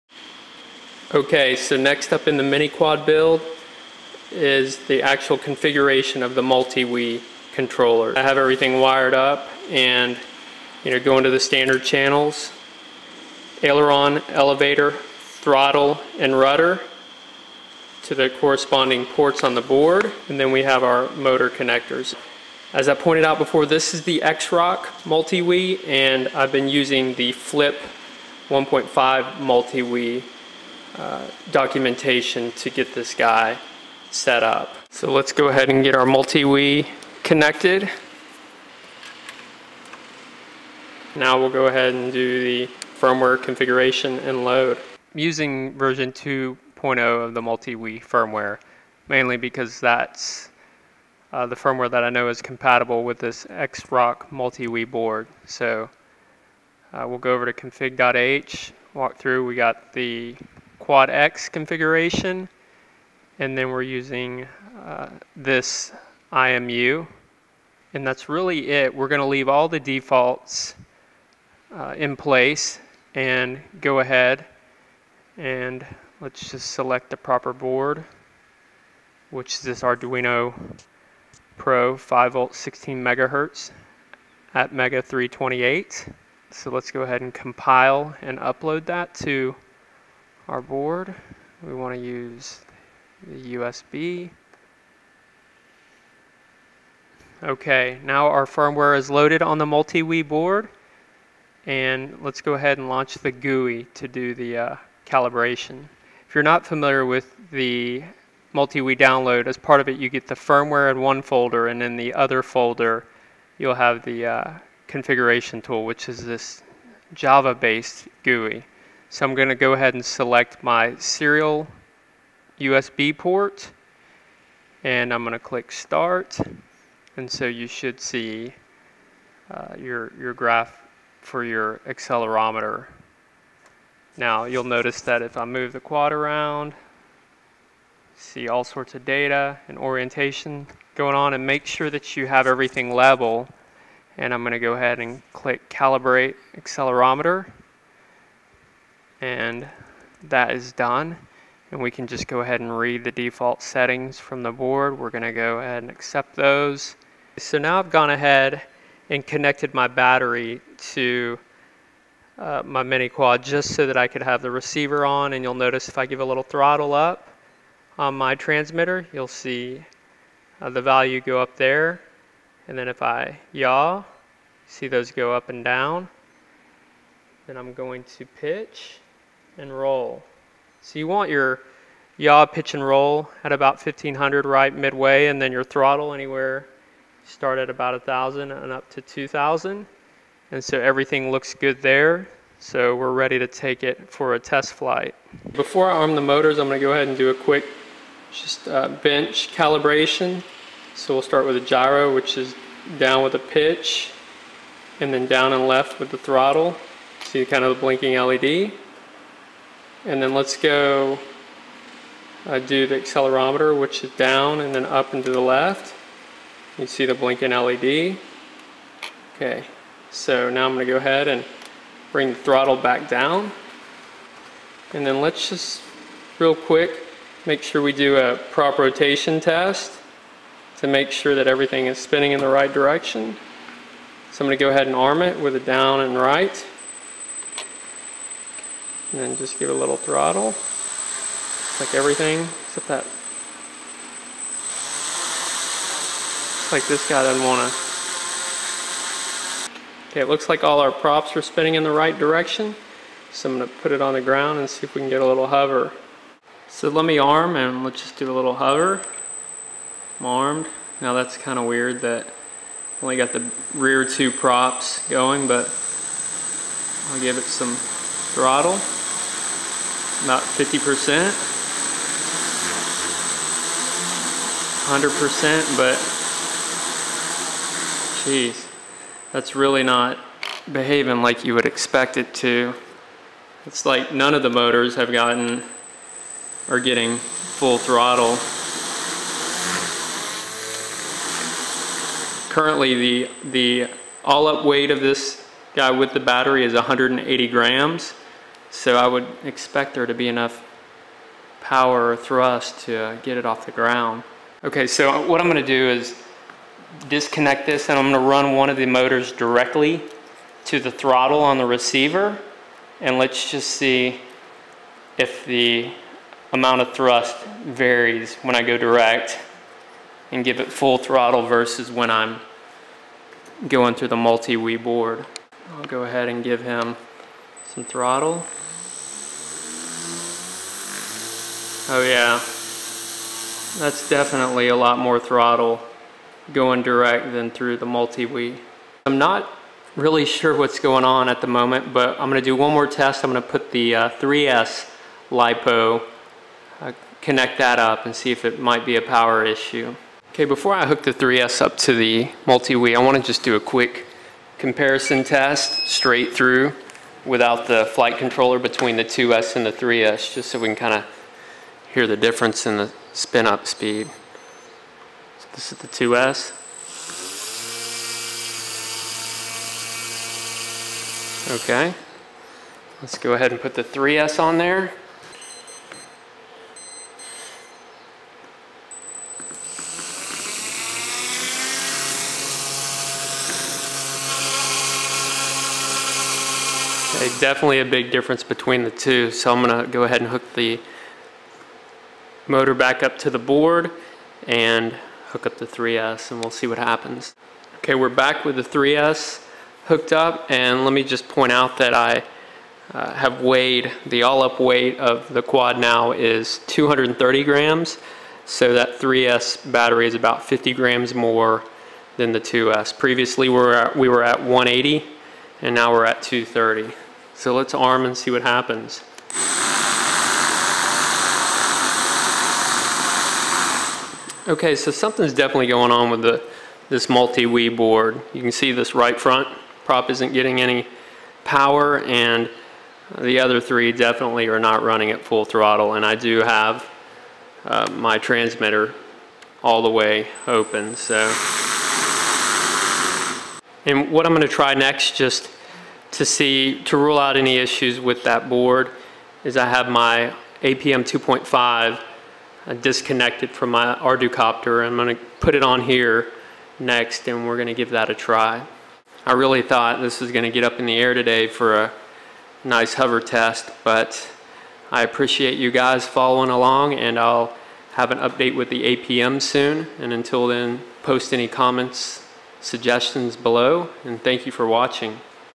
DIY Mini Quadcopter MultiWii Configuration and Maiden Hover Fail
Well it was bound to happen. I was so anxious to get this guy in the air, but it didn't happen. It appears to be a problem with the MultiWii board, but I'm not 100% certain yet. This video walks through the steps for configuring and loading the firmware on your MultiWii. I went with all the basics just so I could get it to hover. If I overlooked something then please let me know.

This was pretty much identical to what I did previously to test the MWC with a 450 quad that I was helping a friend build. Next up I'm going to plug in my APM 2.5 just to see if there's any difference in the way the quad behaves.

Here are my fail theories at the moment:

1. Something misconfigured with the MWC board

2. Motors aren't powerful enough

3. Props too small

The AUW for the quad with the 2s lipo is 180 grams and with the 3s it is 230 grams. I'd have to think there would be enough thrust to get this guy off the ground, but to be honest I have no idea how much thrust they generate. I'm going to do a bench test soon to find out.

If you have any suggestions I'd love to hear them and thanks for following along with this build.